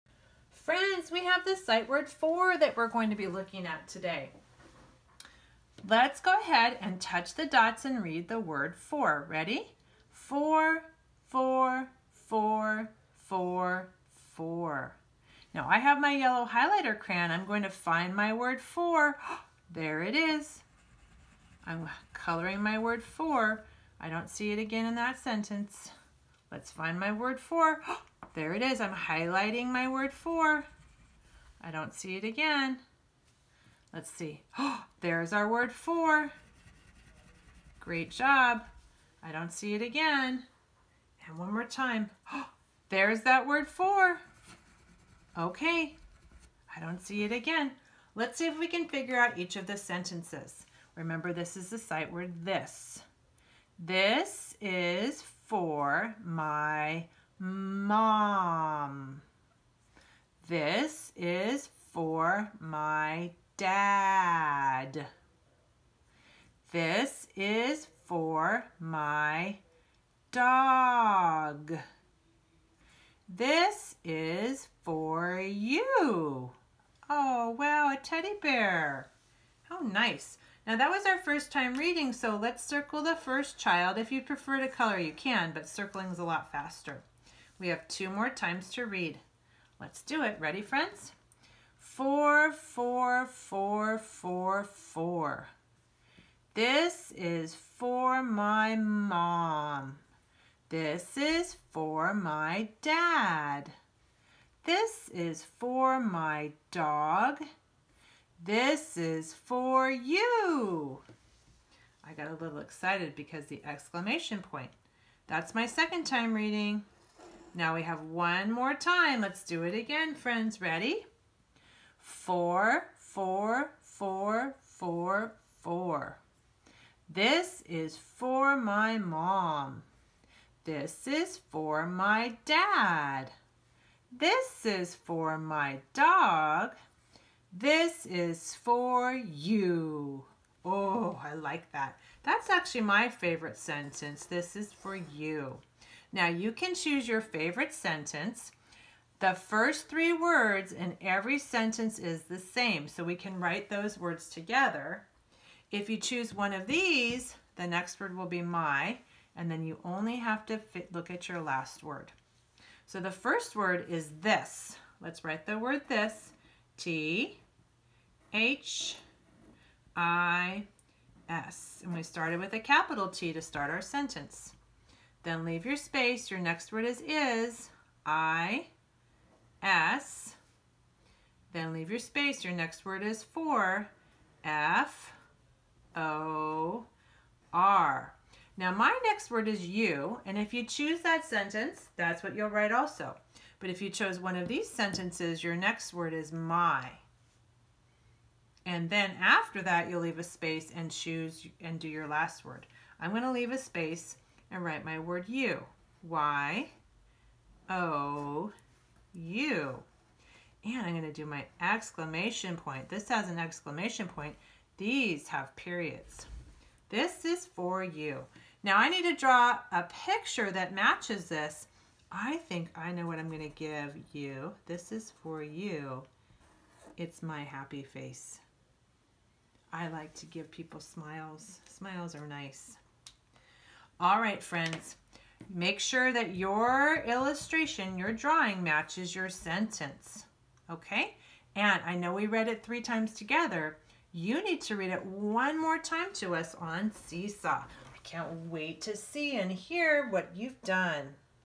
I Can Read! for
Work on reading and writing sight word "for"